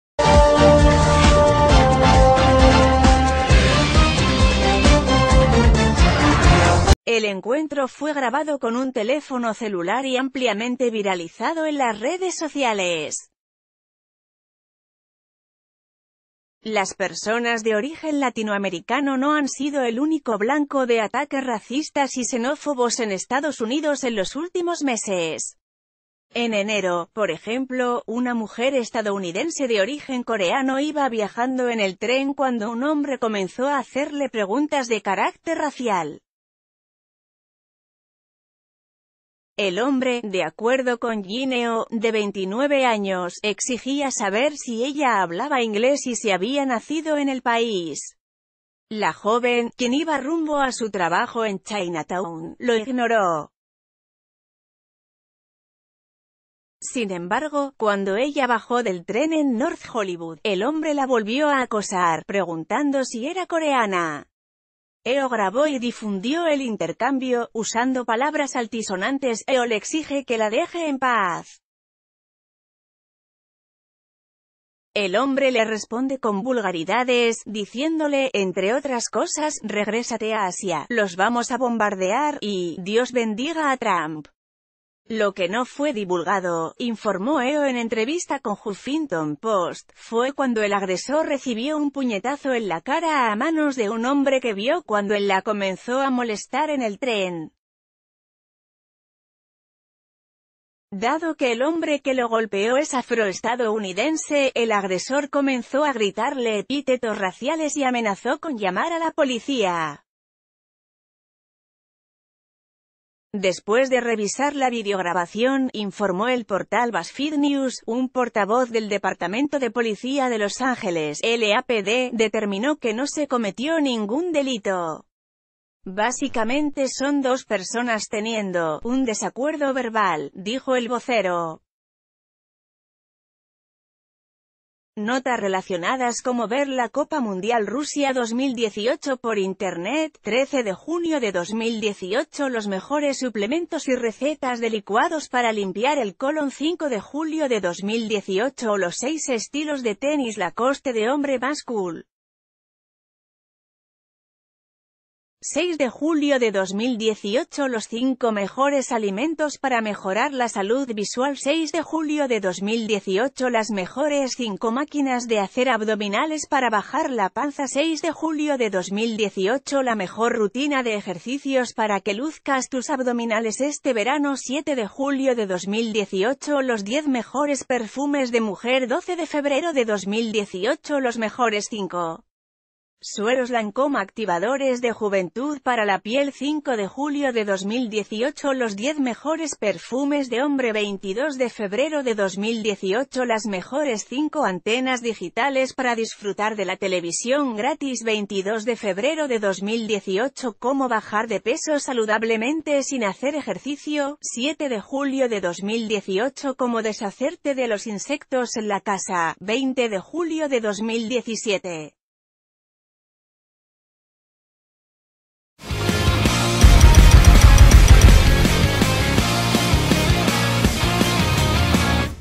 Vídeo: Racista ataca a mujer de origen coreano en Los Ángeles
Las personas de origen latinoamericano no han sido el único blanco de ataques racistas y xenófobos en Estados Unidos en los últimos meses. En enero, por ejemplo, una mujer estadounidense de origen corean...